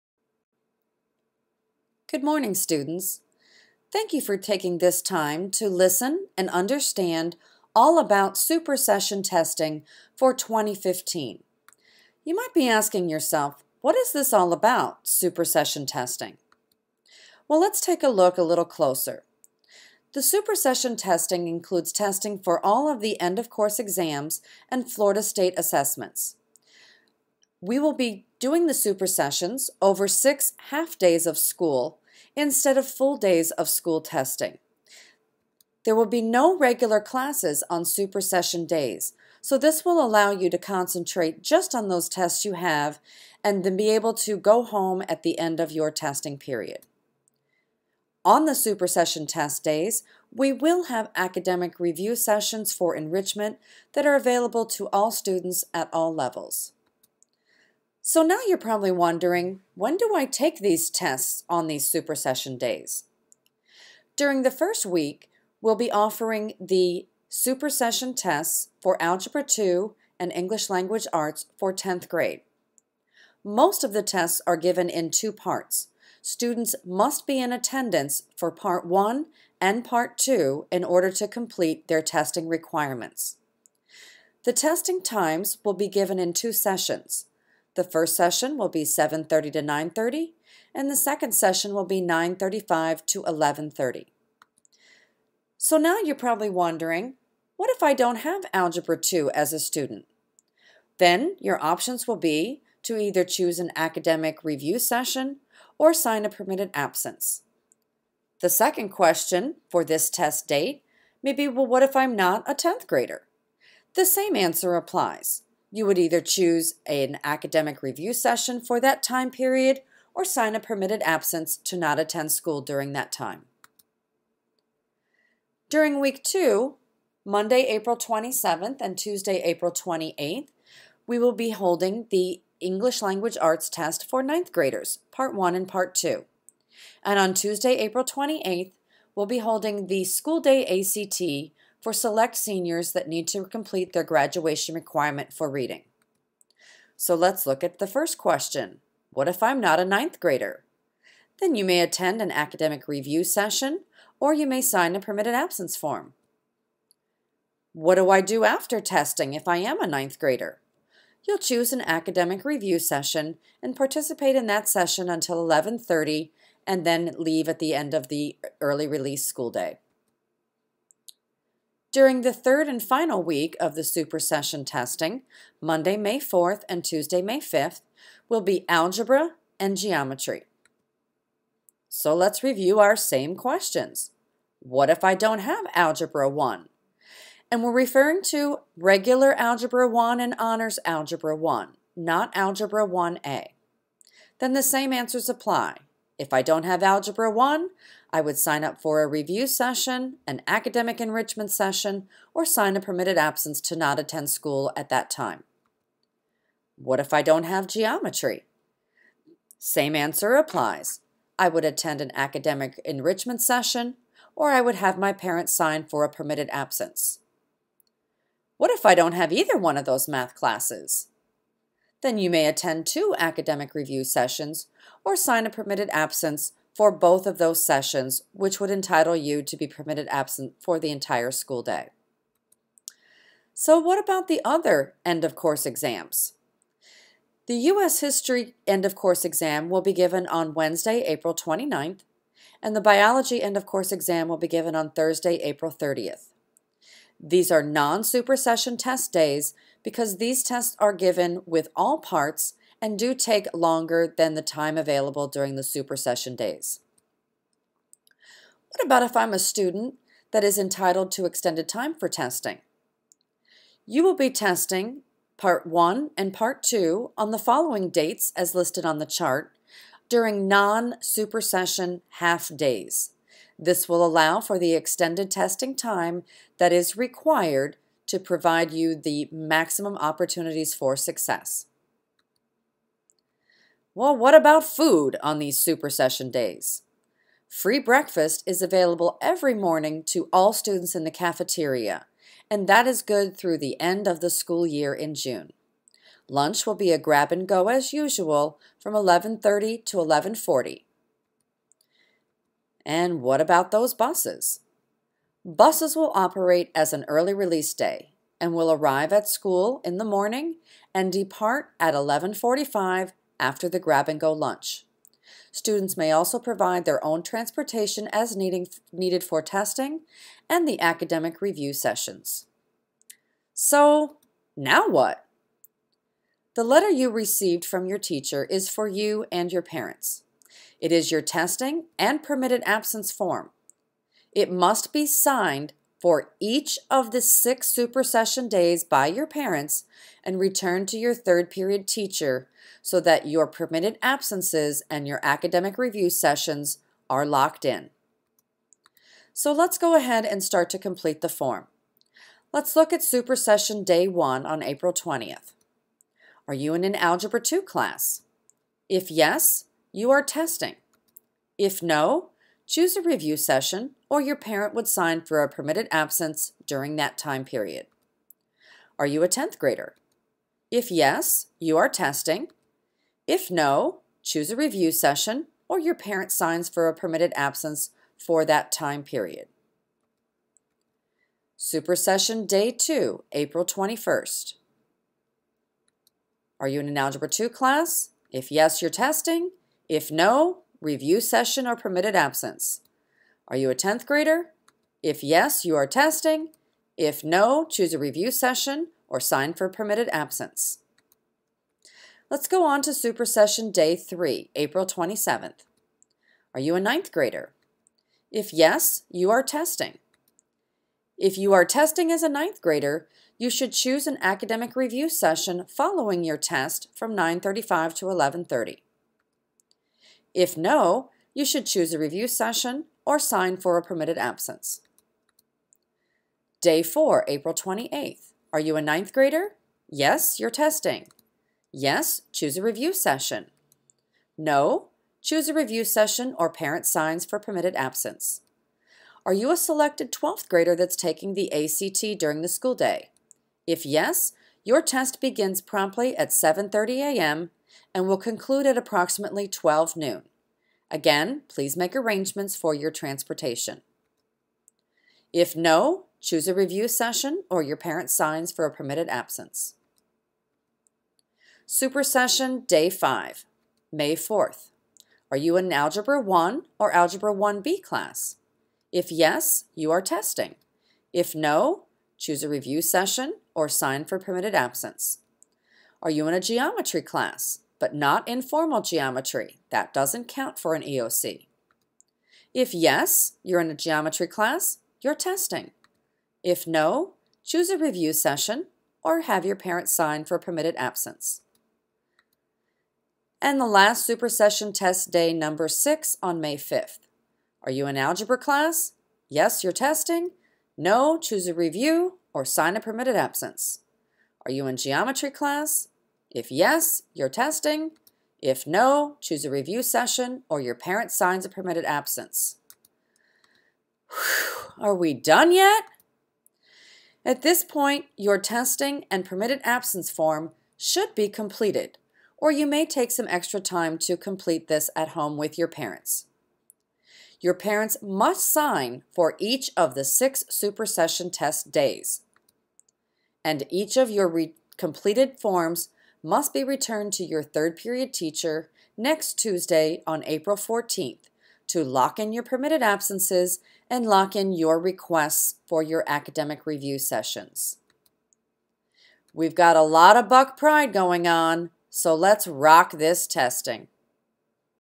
Super Session Testing
This is the How-To video on how you should fill out the Super Session Testing form. This will also answer FAQ's about Super Session Testing and what your options are as a student.